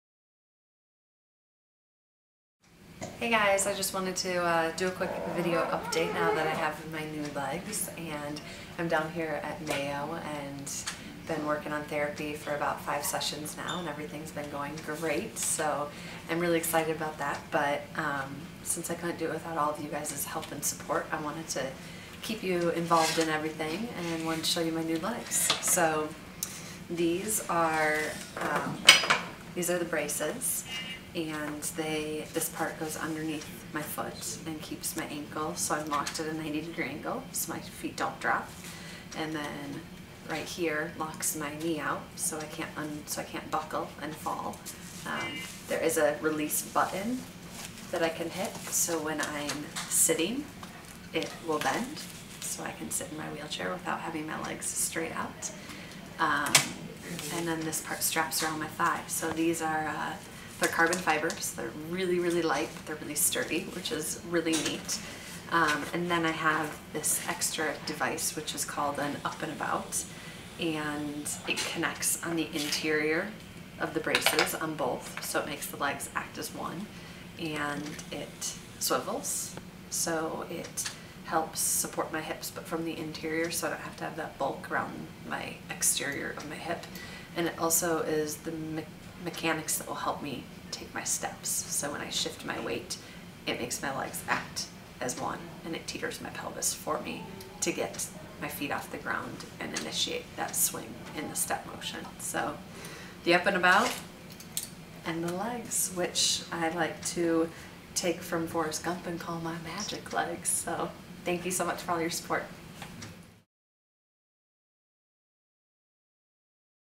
Mallory Weggemann - "The Journey of New Mobility" Part 2
On January 21, 2008 at 18 years old, Mallory Weggemann became paralyzed. Since that day she has dreamed of one day walking again, but in order to make that a reality she needs your help. Please continue to follow Mallory in her dream by visiting the campaign

This video has been put together to continue documenting Mallory's journey towards upward mobility by showcasing her customized leg braces following her recent therapy appointment at Mayo Clinic in Rochester, MN.